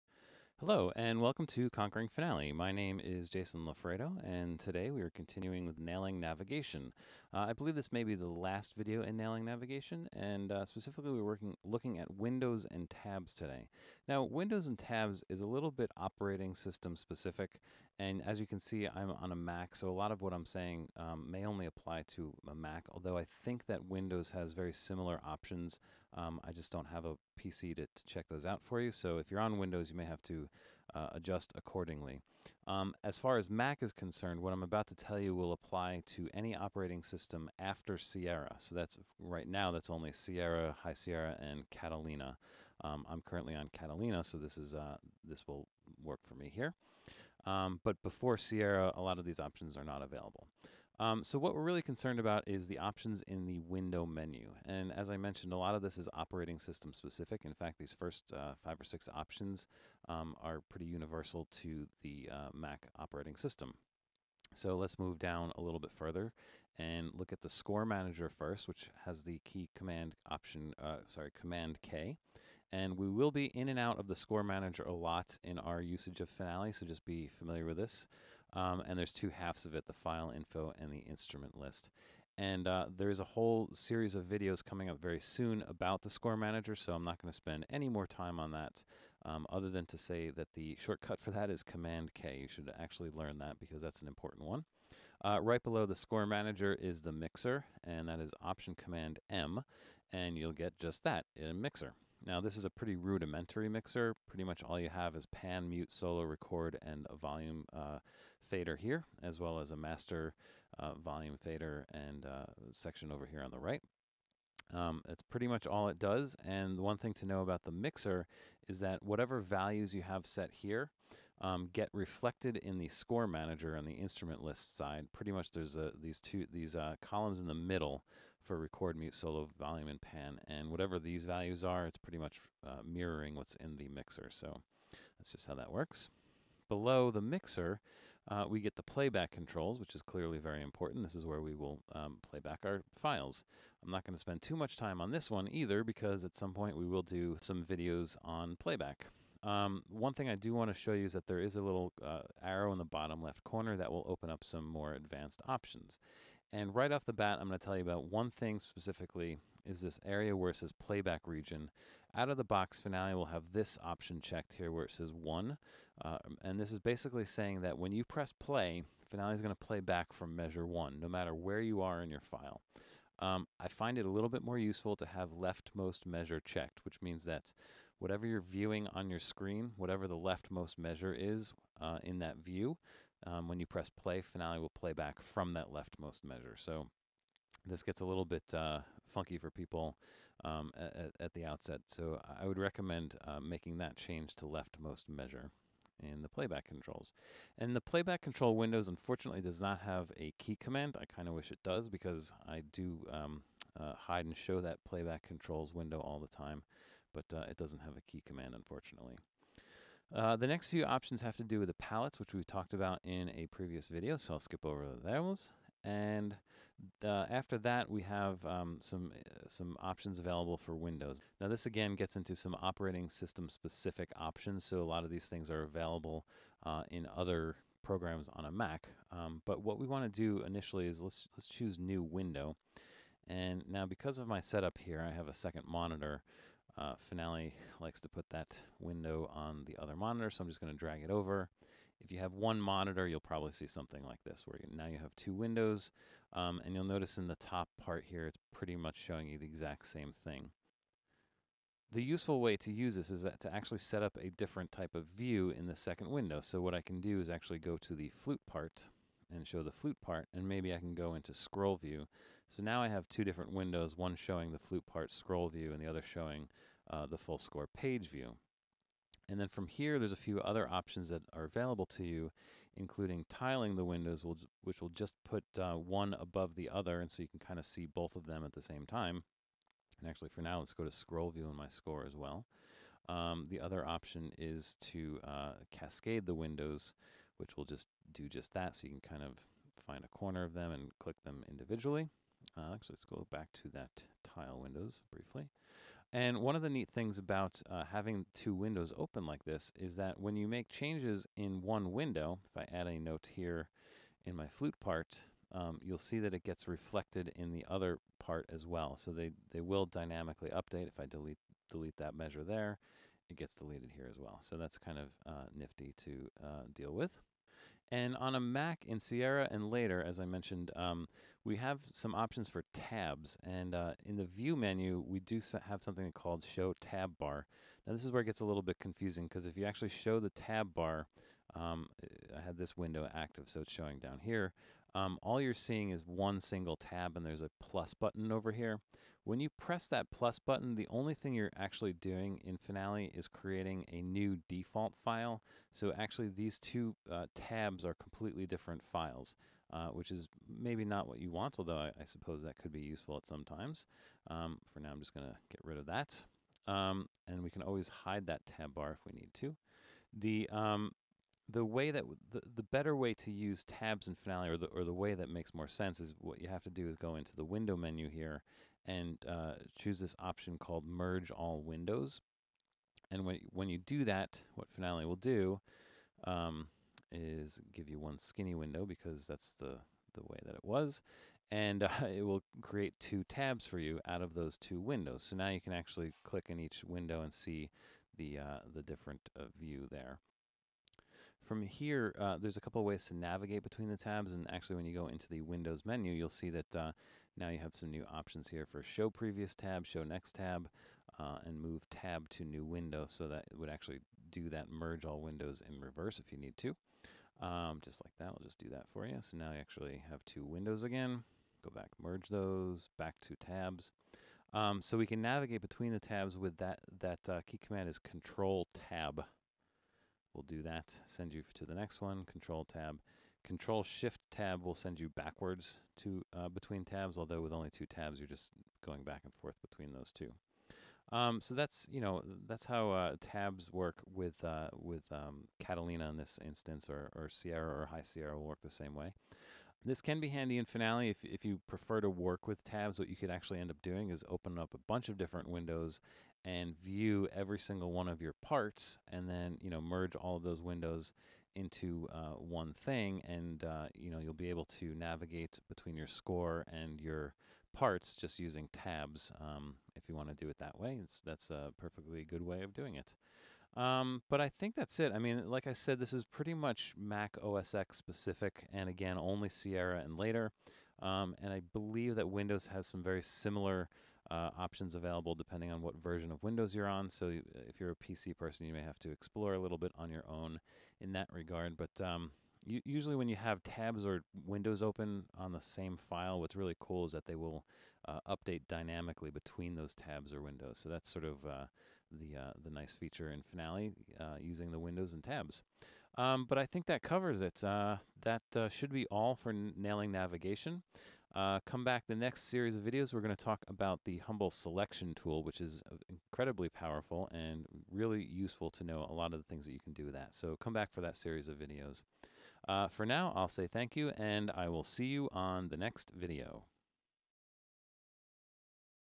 1-6 Nailing Navigation Lesson 6 (Intermediate)
Windows and Tabs
It’s possible to have multiple windows open for the same file in Finale and those windows can also be merged into Tabs if you want. This video will show you how to do all of that. This is one area where Mac and Windows diverge a lot. Since these videos were made on a Mac this lesson may only partially apply to Windows users.
Finale V26.2